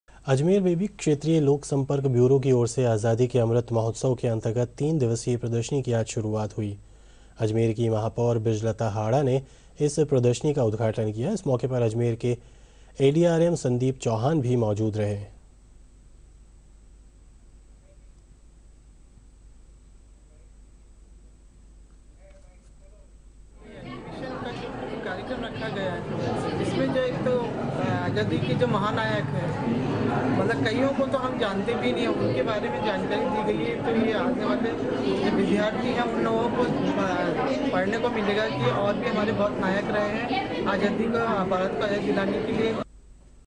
आजादी अमृत महोत्सव : अजमेर
अजमेर में भी क्षेत्रीय लोक संपर्क ब्यूरो की ओर से आजादी के अमृत महोत्सव के अंतर्गत तीन दिवसीय प्रदर्शनी की आज शुरूआत हुई। अजमेर की महापौर बृजलता हाडा ने प्रदर्शनी का उदघाटन किया। इस मौके पर अजमेर के एडीआरएम संदीप चौहान भी मौजूद रहे।